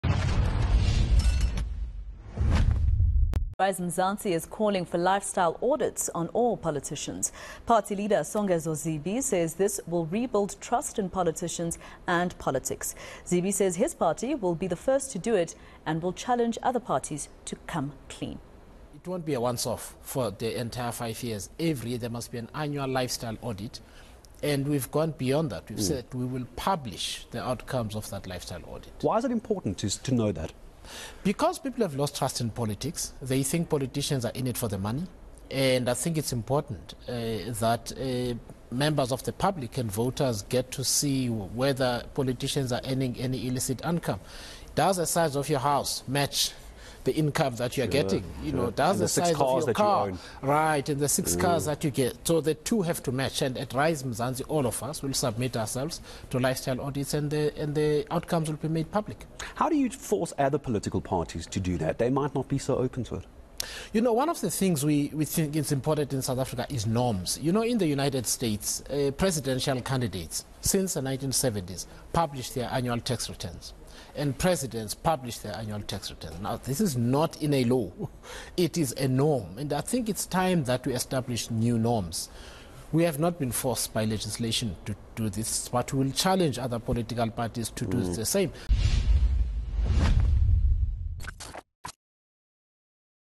Rise Mzansi is calling for lifestyle audits on all politicians
Rise Mzansi is calling for lifestyle audits on all politicians. Party leader Songezo Zibi says this will rebuild trust in politicians and politics.